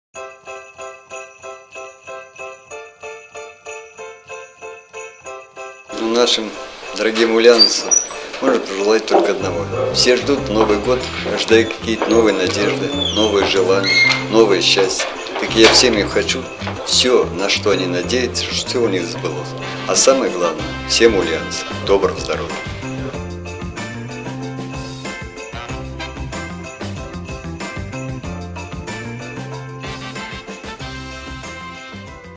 Новогоднее поздравление депутат ЗСО А. Букина
овогоднее поздравление депутата Законодательного собрания Ульяновской области Александра Букина